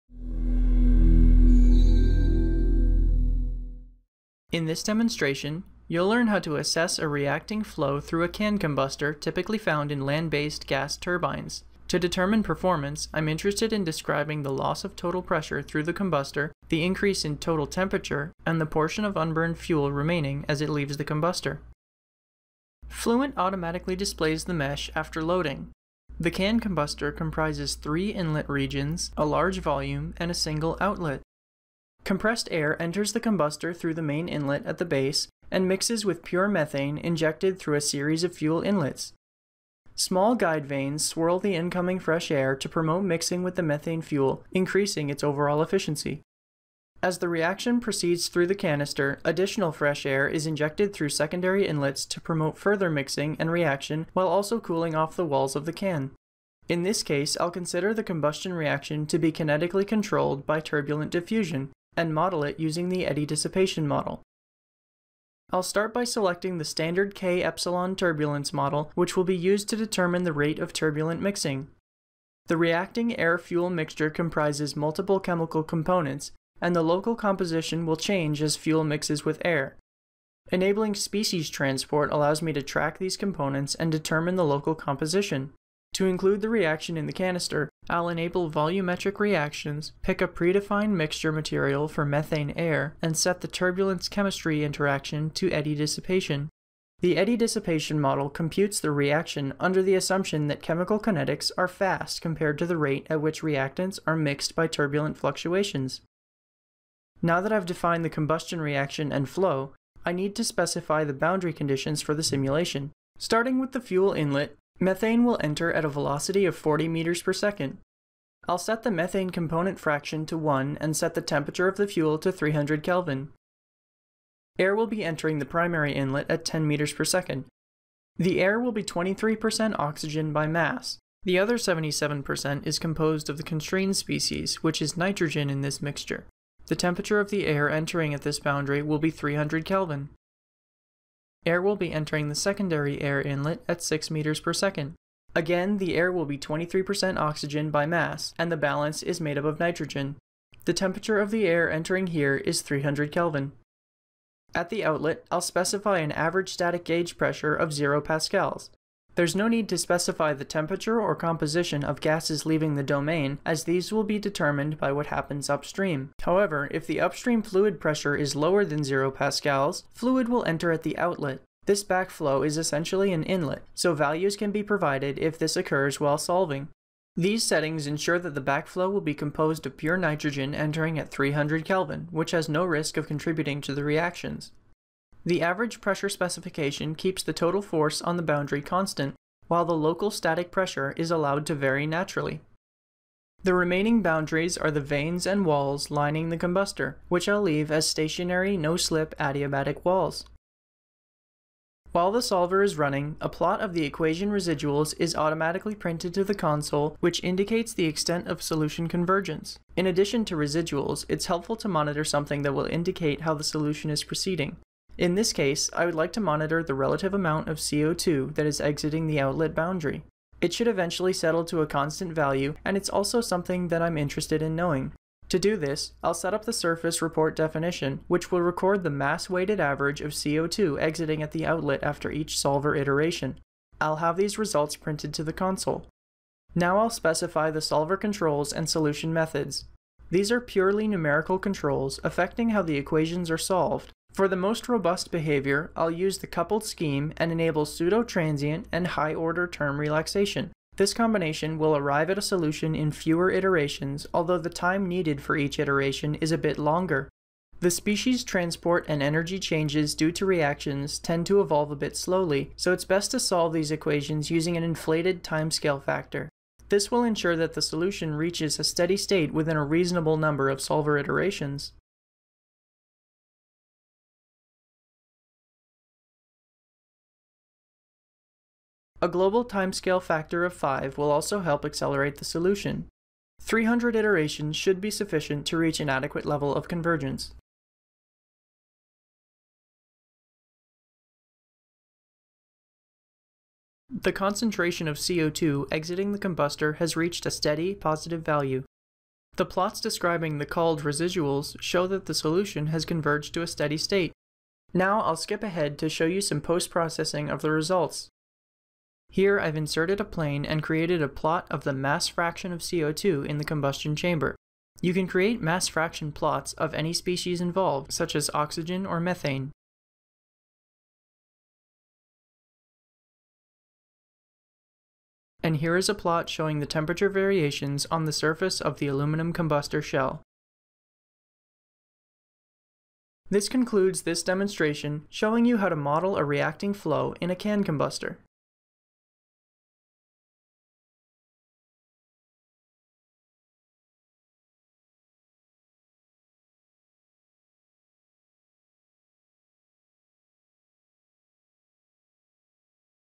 ANSYS Fluent: Diffusion Controlled Reacting Flow in a Can Combustor
This video demonstrates how to set up a reacting flow simulation in ANSYS Fluent for a can combustor using the eddy dissipation model.